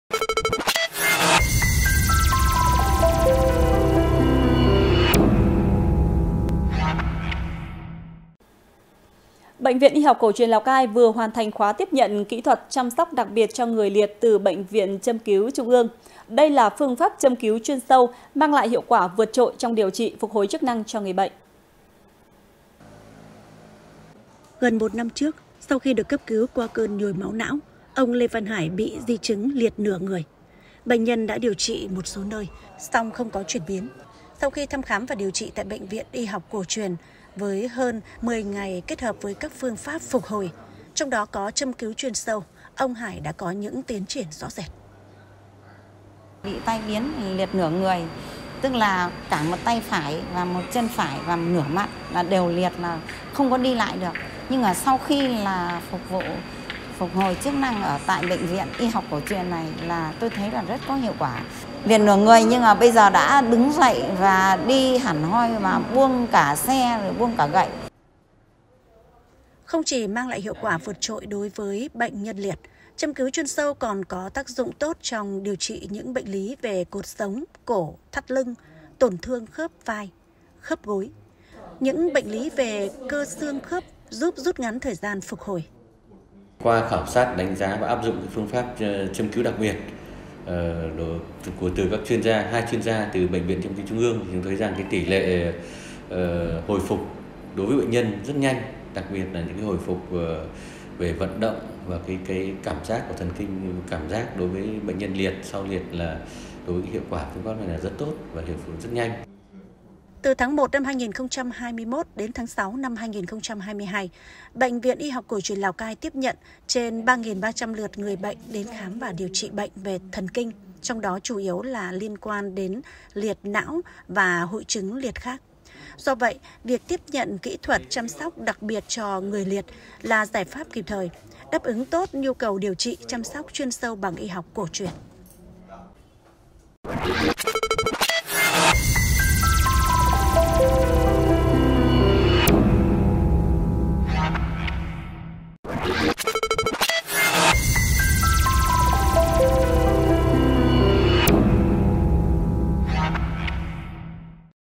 Ứng dụng hiệu quả phương pháp châm cứu mới trong điều trị bệnh nhân bị liệt| THLC
Laocaitv.vn - Bệnh viện Y học cổ truyền Lào Cai vừa hoàn thành khoá tiếp nhận kỹ thuật "Chăm sóc đặc biệt cho người liệt" từ Bệnh viện Châm cứu Trung ương. Đây là phương pháp châm cứu chuyên sâu, mang lại hiệu quả vượt trội trong điều trị, phục hồi chức năng cho người bệnh.
Đài Phát thanh - Truyền hình Lào Cai